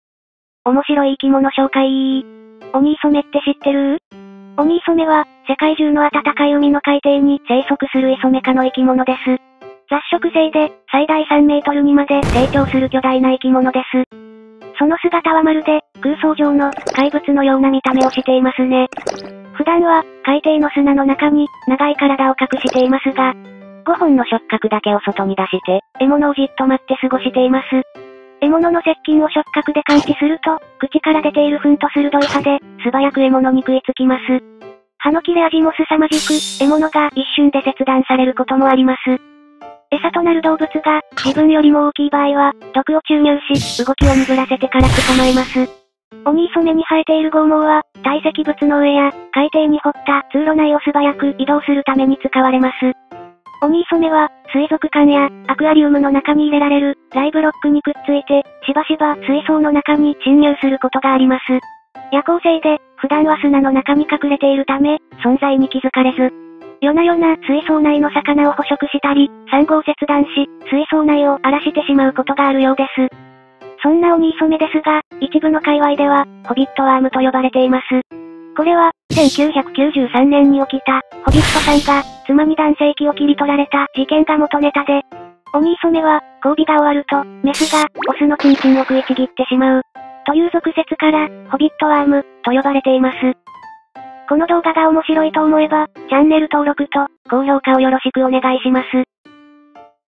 最大3メートルのデスワーム 生態解説【オニイソメ】面白い生き物紹介
オニイソメは世界中の暖かい海の、海底に生息するイソメ科の生き物です。その姿はまるで空想上の怪物のような見た目をしていますね。

普段は海底の砂の中に長い体を隠していますが、獲物の接近を触角で感知すると、素早く獲物に食いつきます。

オニイソメは交尾が終わると、メスがオスのチン○ンを食いちぎってしまうという俗説から、ホビットワームと呼ばれています。

使用させていただいた動画のリンクは以下のとおりです。